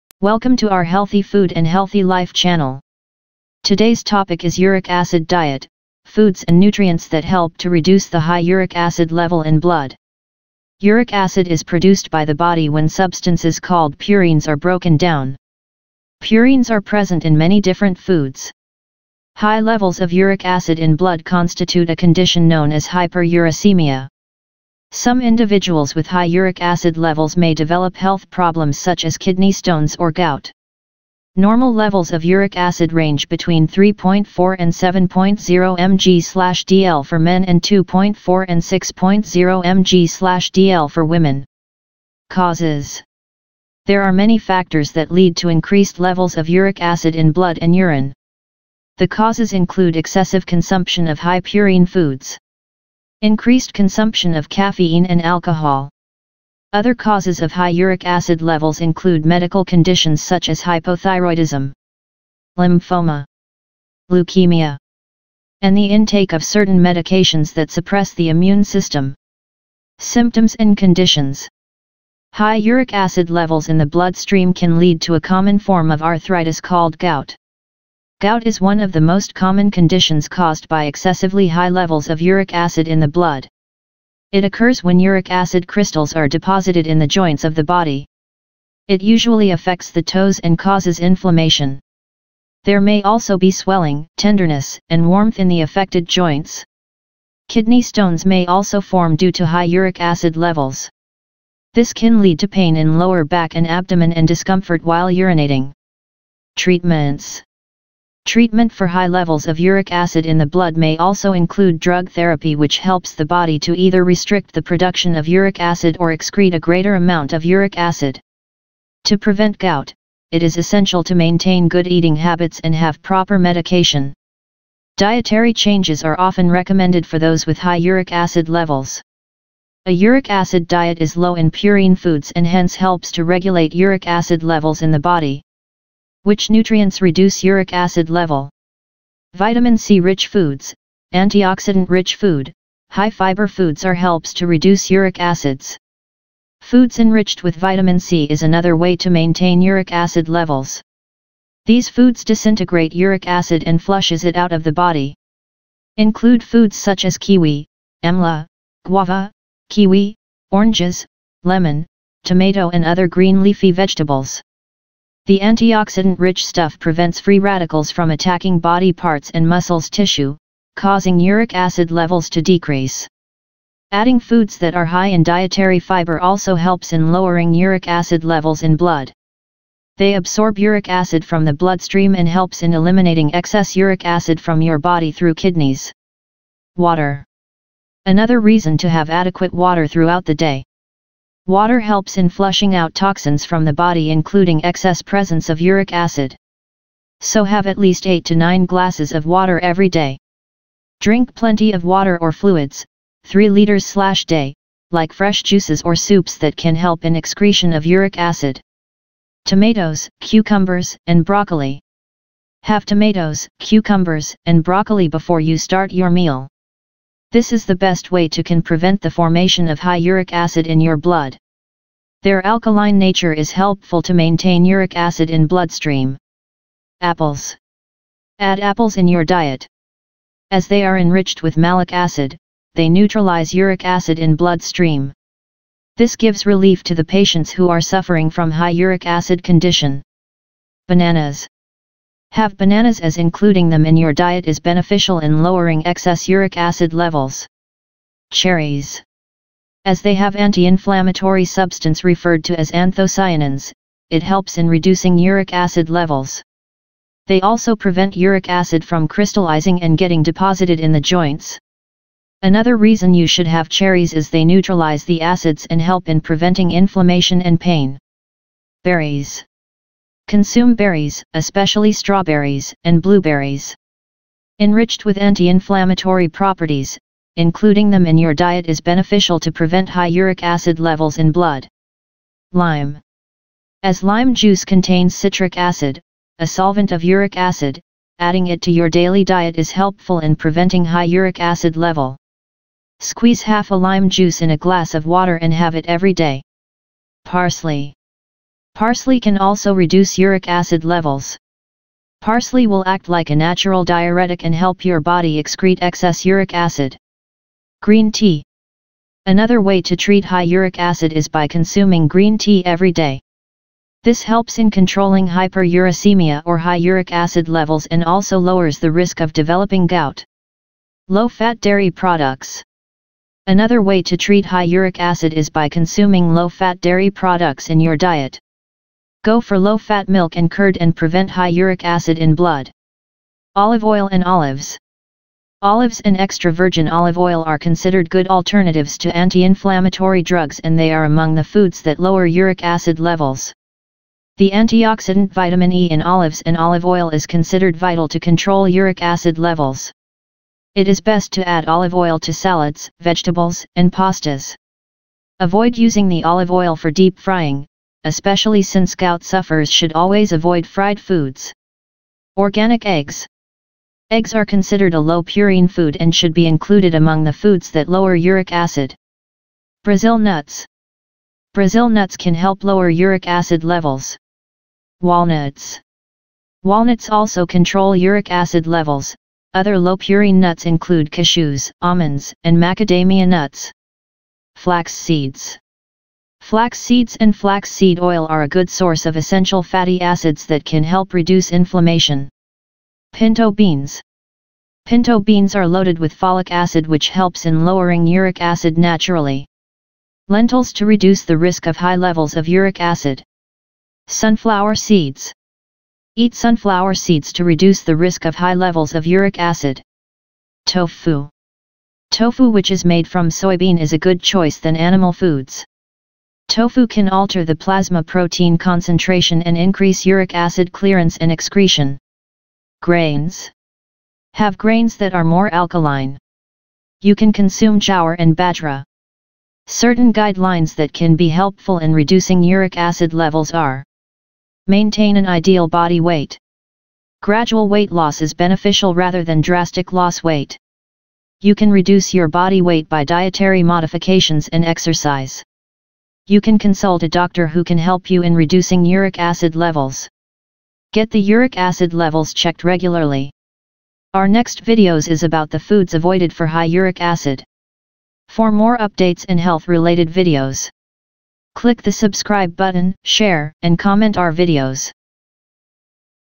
Uric Acid Diet: Uric acid reduction diet. Uric acid reducing foods and nutrients.Gout reducing foods
Uric Acid Diet: Foods and nutrients that help to reduce the high Uric acid level in the blood.
Uric acid is produced by the body when substances called purines are broken down. Purines are present in many different foods. High levels of uric acid in the blood constitute a condition known as hyperuricemia. Some individuals with high uric acid levels may develop health problems such as kidney stones or gout.
Normal levels of uric acid range between 3.4 and 7.0 mg/dL for men and 2.4 and 6.0 mg/dL for women.